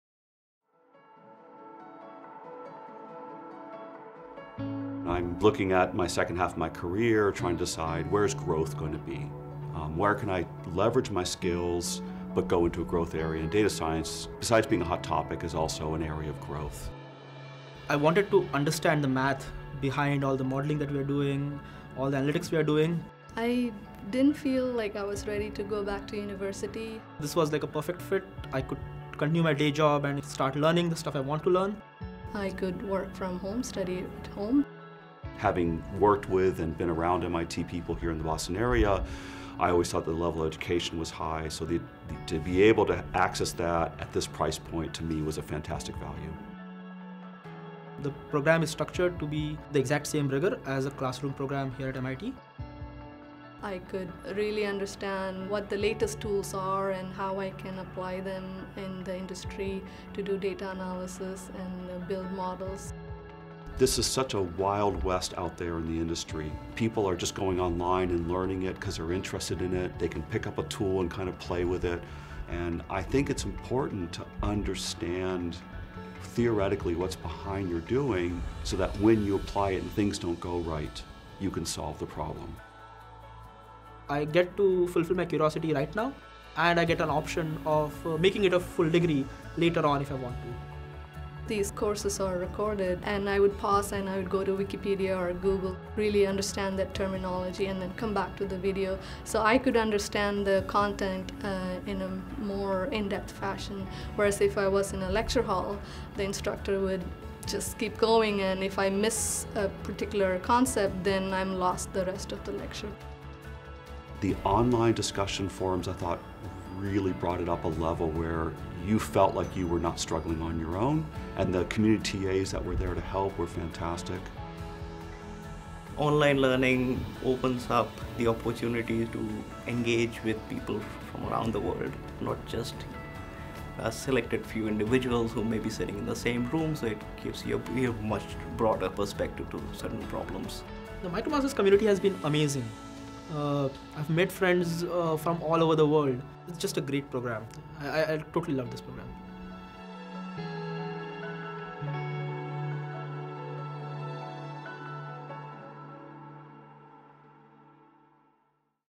MITx MicroMasters® Program in Statistics and Data Science | Learner Testimonials
What are learners saying about the MITx MicroMasters program in Statistics and Data Science? Four learners share their experiences in this video.